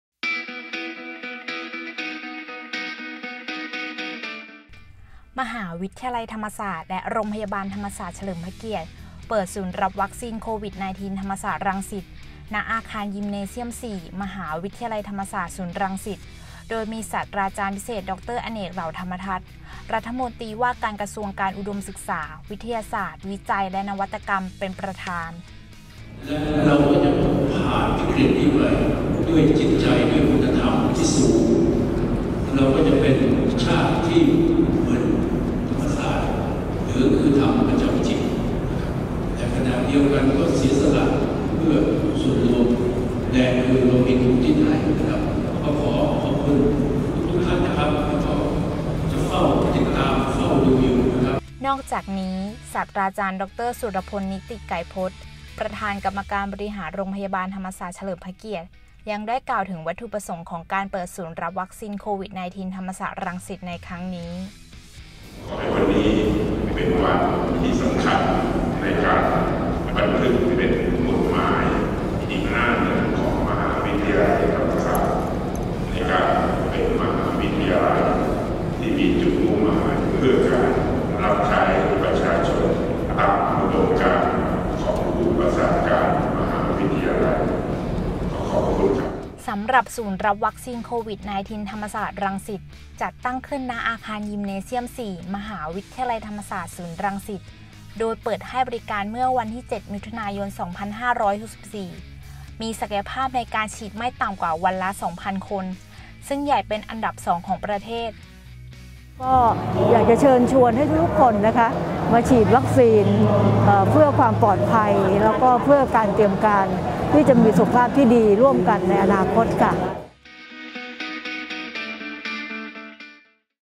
ธรรมศาสตร์ และโรงพยาบาลธรรมศาสตร์ฯ เปิด "ศูนย์รับวัคซีนโควิด-19 ธรรมศาสตร์รังสิต"
มหาวิทยาลัยธรรมศาสตร์ และโรงพยาบาลธรรมศาสตร์เฉลิมพระเกียรติ เปิด “ศูนย์รับวัคซีนโควิด-19 ธรรมศาสตร์รังสิต” ณ อาคารยิมเนเซียม 4 มหาวิทยาลัยธรรมศาสตร์ ศูนย์รังสิต
อ่านข่าวต่อได้ที่